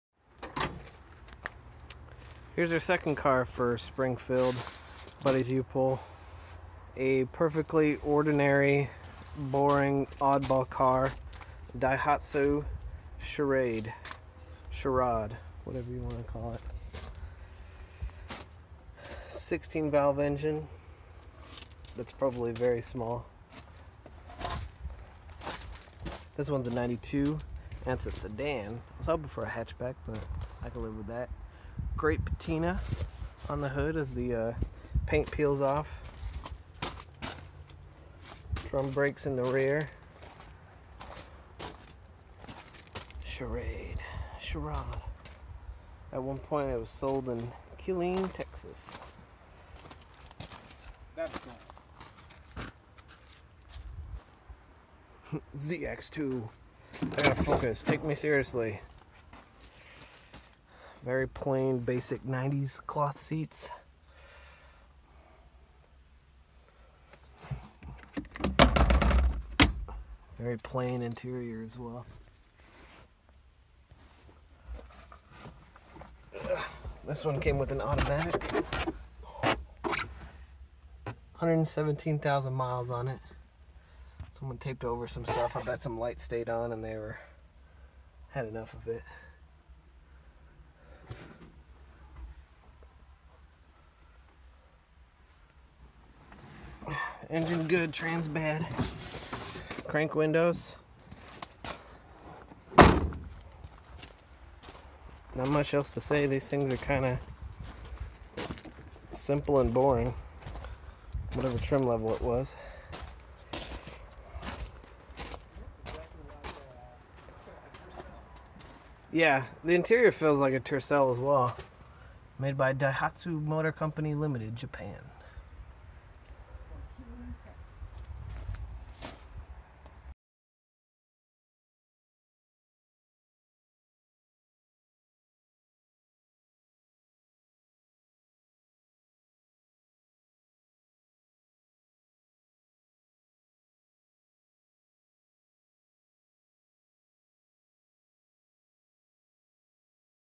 Junkyard Finds: Daihatsu Charade | Mesquite Automotive
Another bucket list car is the 1 of only 2 US market vehicles from Daihatsu. Turns they're actually really plain and boring. Hopefully if I ever find a Rocky it will be a little more interesting.

Please like Mesquite Automotive on facebook!